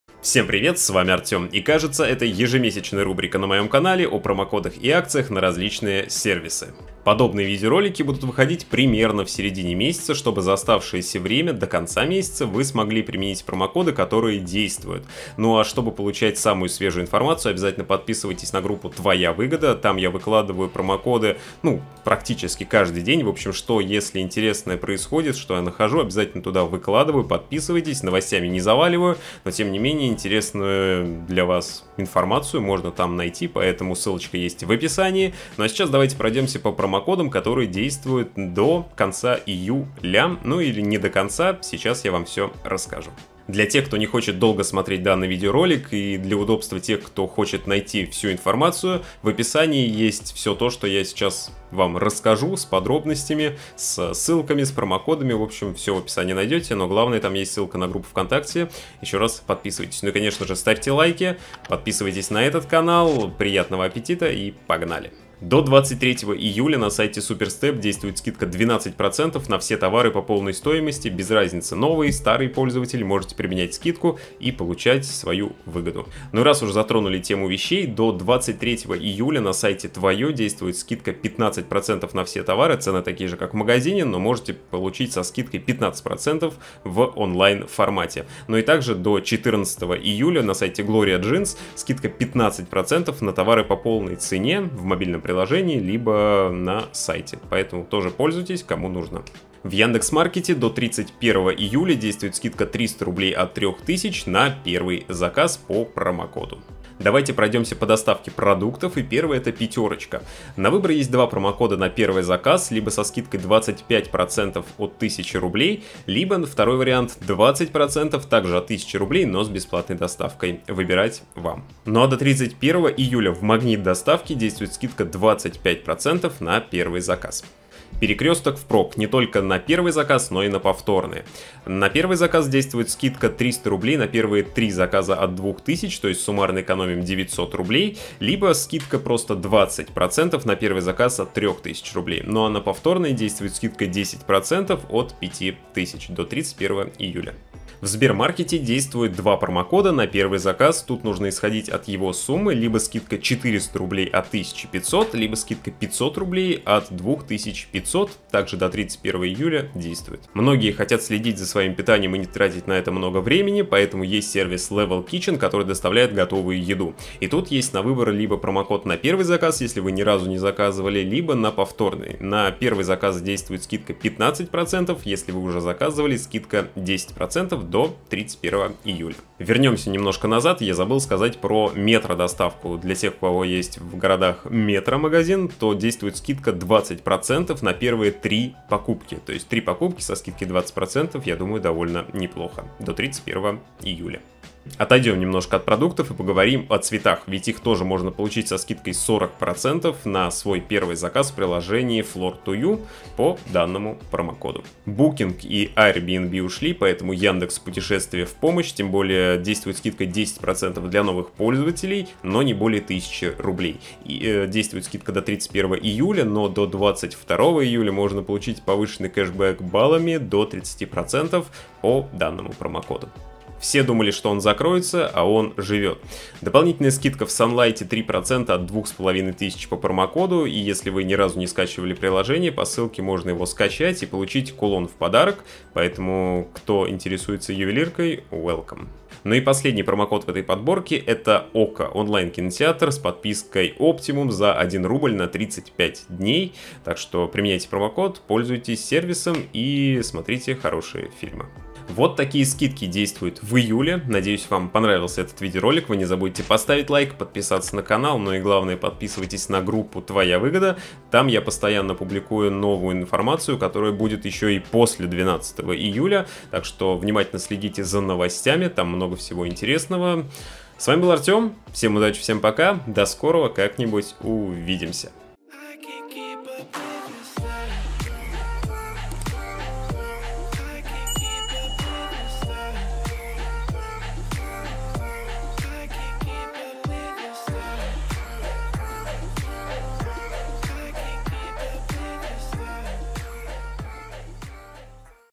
ПРОМОКОДЫ ИЮЛЬ 2022 - ЯНДЕКС МАРКЕТ, СБЕР МАРКЕТ, МАГНИТ, ПЯТЁРОЧКА, ПЕРЕКРЕСТОК, ТВОЁ И ДРУГИЕ
Всем привет! В этом ролике я собрал для вас подборку промокодов на различные сервисы которые работают в июле 2022 года, успейте получить скидку!

⭐️ До 23 июля. SuperStep. Скидка 12% на все товары по полной цене на сайте SuperStep.
🏷 Промокод: SL48DC6

⭐️ До 24 июля. Unisonshop. Скидка 15% на весь ассортимент Unisonshop при заказе от 2000 рублей.
🏷 Промокод: GB7133

⭐️ До 23 июля. ТВОЕ. Скидка 15% на сайте «ТВОЕ»
🏷 Промокод: 2131Y

⭐️ До 31 июля. Автор 24. Получи 550 рублей по промокоду на первый заказ в мобильном приложении «Автор 24»
🏷 Промокод: GB-95533

⭐️ До 14 июля. Gloria Jeans. Скидка 15% на все товары по полной цене в интернет-магазине и мобильном приложении Gloria Jeans!
🏷 Промокод: 01-GJEC8

⭐️ До 31 июля. Яндекс Маркет. Скидка 300 рублей на первый заказ при покупке от 3000 рублей по промокоду в Яндекс.Маркет
🏷 Промокод: TRB72-AF

⭐️ До 31 июля. METRO. Скидка 20% на первые ТРИ покупки в METRO Cash&Carry
🏷 Промокод: f98m4

⭐️ До 31 августа. Пятёрочка доставка. Скидка до 25% в приложении (один из двух промокодов)
🏷 Промокод: PF2040 - скидка 25% на первый заказ от 1000 рублей
🏷 Промокод: DV2038 - скидка 20% на первый заказ от 1000 рублей + бесплатная доставка

⭐️ До 31 июля. Магнит доставка. Скидка 25% по промокоду на первый заказ
🏷 Промокод: SUNNY23533

⭐️ До 31 июля. Level Kitchen. Скидка 15% для новых пользователей и скидка 10% на повторный заказ.
🏷 Промокод: LNM539 - скидка 15% на первый заказ
🏷 Промокод: LDMf4z - скидка 10% на повторный заказ

⭐️ До 31 июля. Перекрёсток впрок. Скидки на первый и повторный заказы.
🏷 Промокод: 3pkcgd - скидка 300₽ на первые ТРИ заказа от 2000₽.
🏷 Промокод: 20pfx5 - скидка 20% на первый заказ от 3000₽.
🏷 Промокод: 1pkp91 - скидка 10% на все повторные заказы от 5000₽.

⭐️ До 31 июля. Flor2u. Скидка 40% на первый заказ цветов в мобильном приложении.
🏷 Промокод: FJLRS7879

⭐️ До 31 июля. Сбермаркет. Скидка на первый заказ (один из предложенных промокодов исходя от суммы заказа)
 🏷 Промокод: gwvhf4 - скидка 400₽ от 1500₽
🏷 Промокод: gbgumd - скидка 500₽ от 2500₽

⭐️ До 31 июля. Яндекс Путешествия. Скидка 10% от любой суммы для новых пользователей. Максимальная скидка 1000 рублей!
🏷 Промокод: PERFL-RWPKU

⭐️ До 31 июля. SUNLIGHT. Дополнительная скидка 3% на весь ассортимент от 2500 рублей по промокоду + кулон в подарок при первом скачивании приложения
🏷 Промокод: бриллиант3171

🏷 Промокод: 43184966